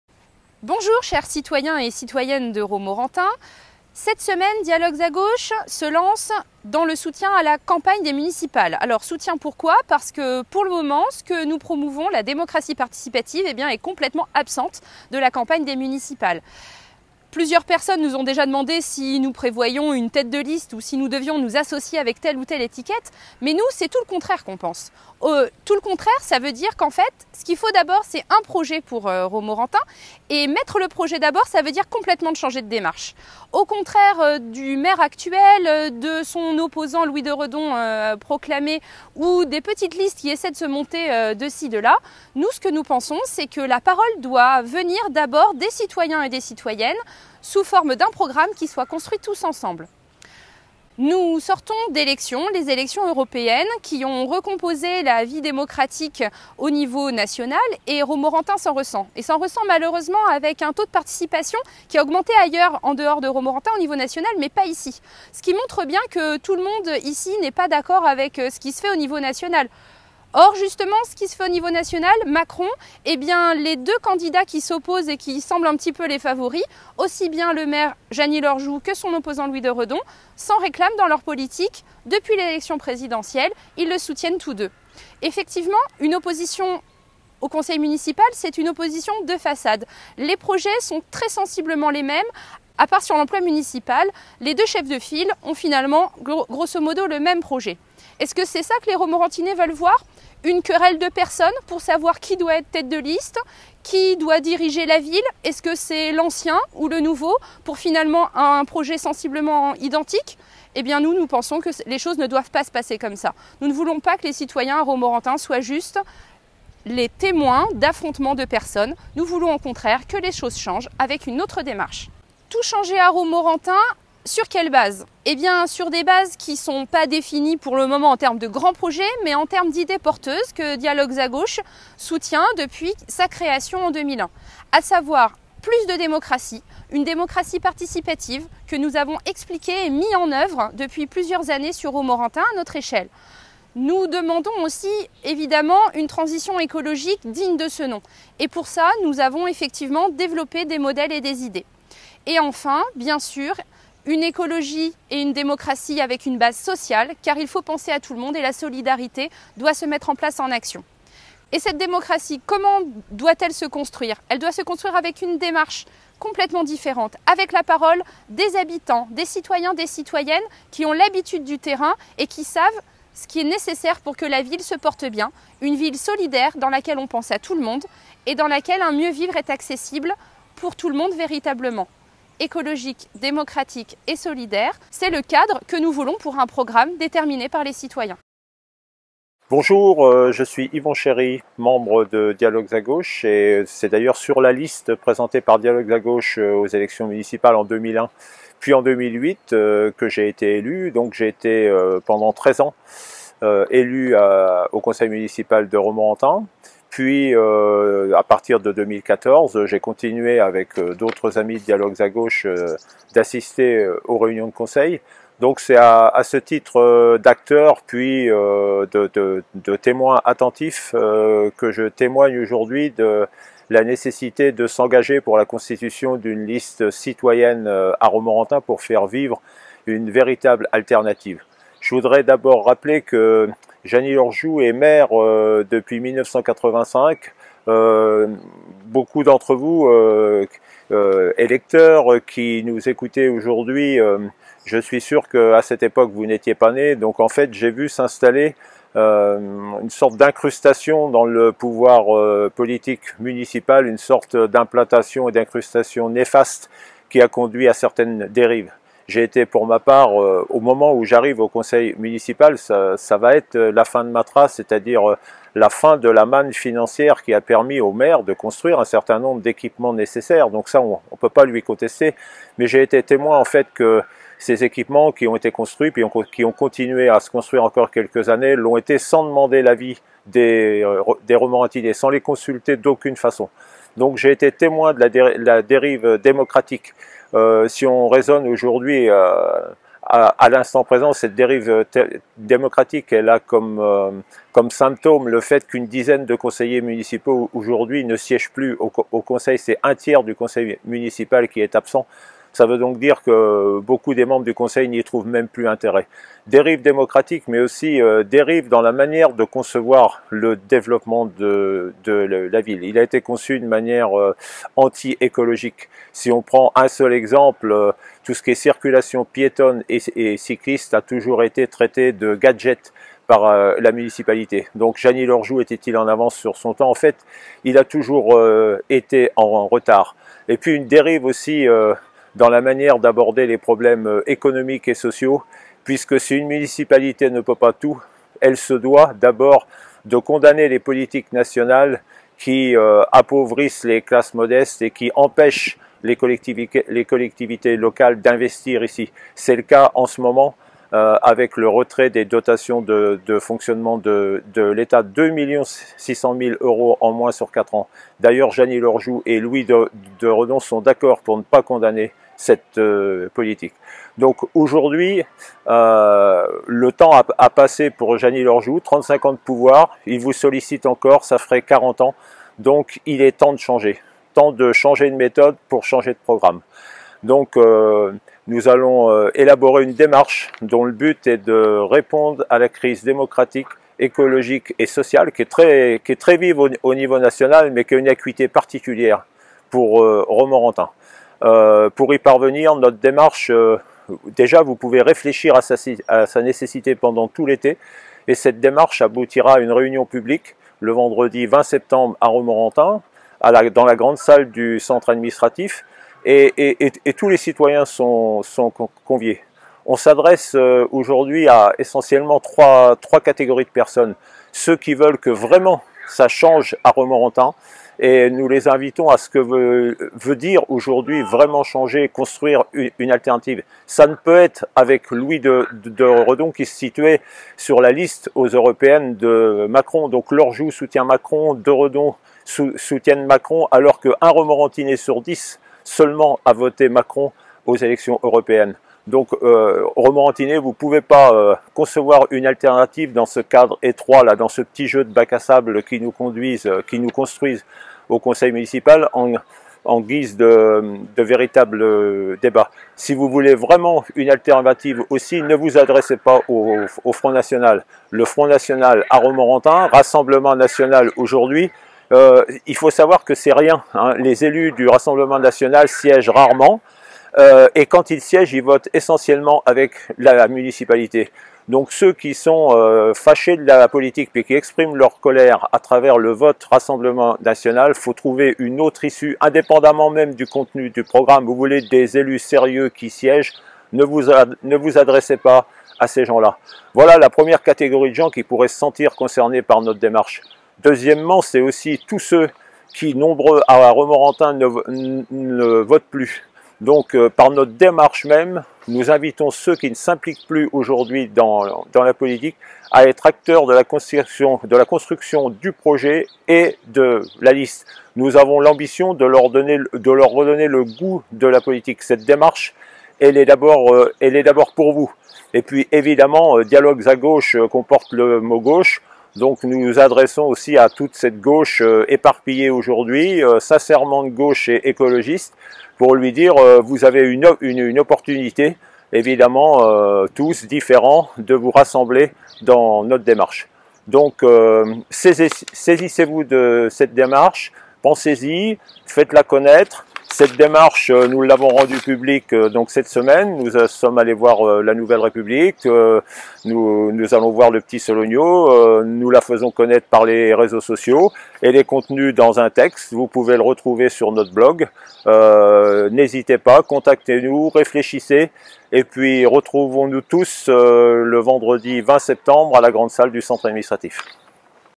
Déclaration d'intention pour 2020
Réunion publique le vendredi 20 septembre à 20h au centre administratif.
Dialogues à Gauche prend l'initiative d'une liste citoyenne. Les citoyens intéressés d'abord, mais aussi les structures politiques, syndicales et associatives, qui le seraient également, sont conviés à construire liste et programme ensemble, puis dans un deuxième temps à les soutenir s'ils s'y reconnaissent.